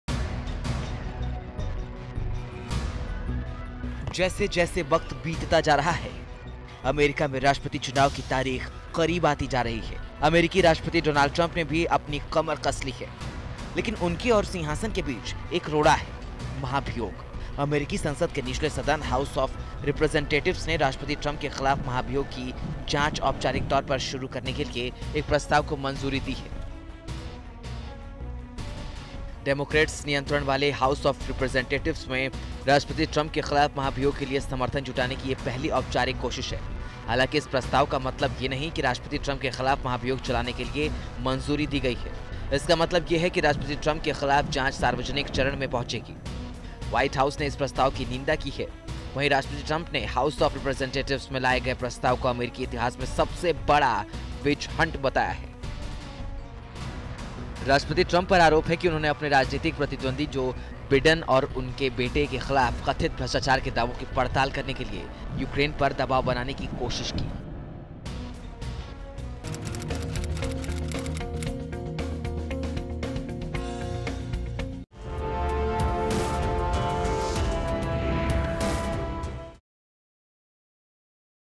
TRUMP पर मंडरा रहा सबसे बड़ा खतरा !
डोनाल्ड ट्रंप को हटाए जाने वाली महाभियोग की प्रक्रिया अब एक कदम आगे बढ़ सकती है, इसपर ट्रंप ने ट्वीट कर विपक्षी पार्टीयों पर निशाना साधा है. लेकिन इसी के साथ सवाल ये भी उठता है कि क्या डोनाल्ड ट्रंप को हटाया जाना संभव है?